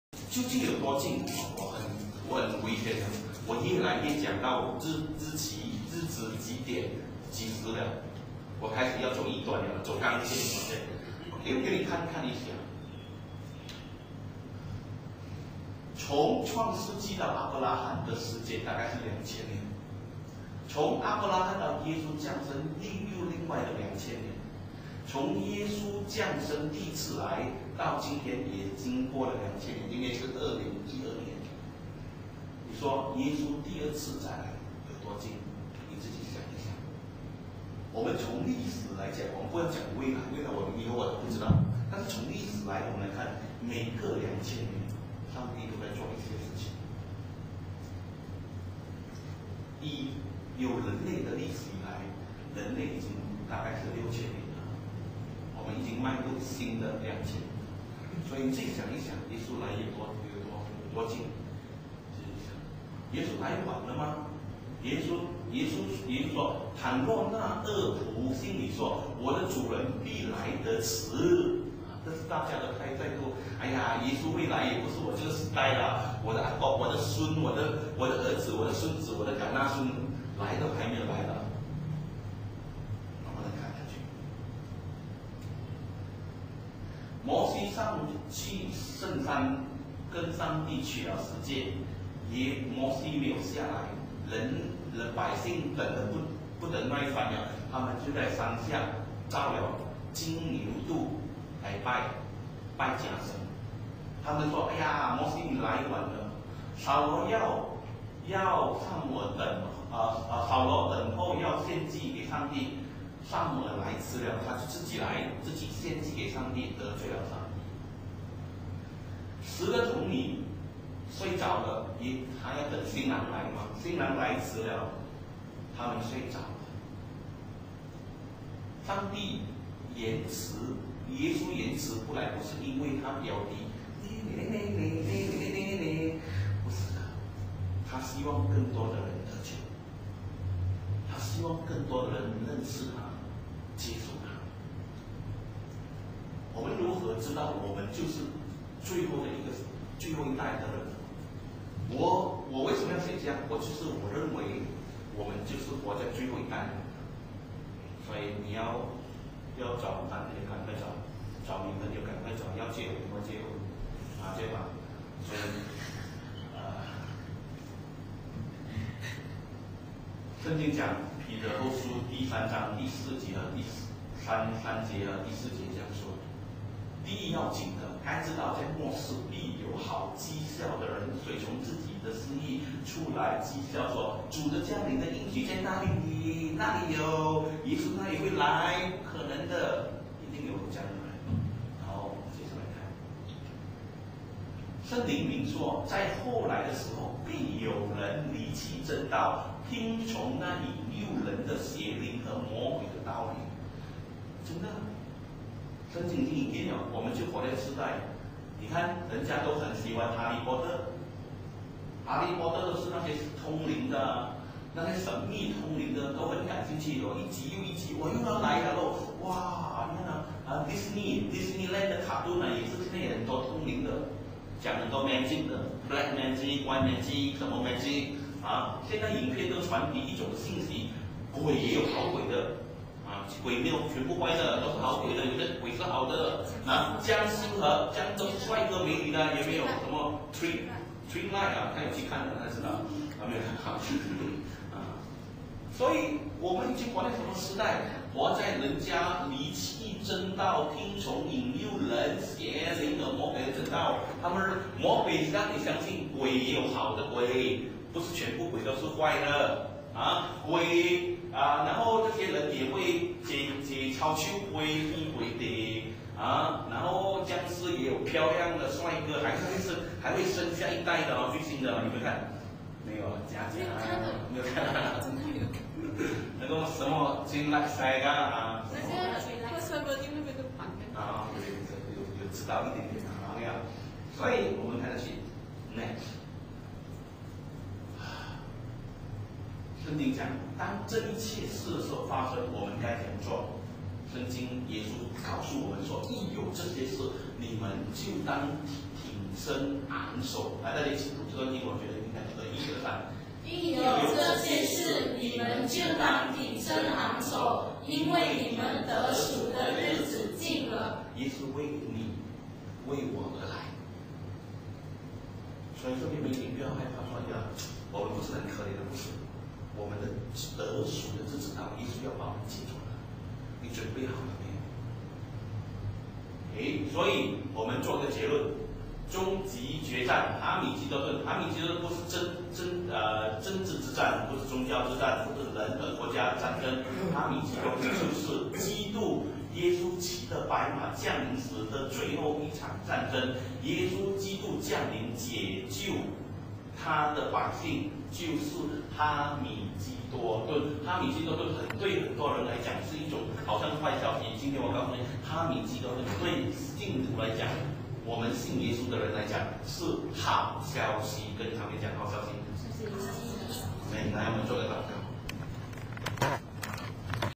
日子近了，究竟有多近？
《终极决战——哈米吉多顿》讲道精华
曾仪生牧师证道
2012年11月11日，星期天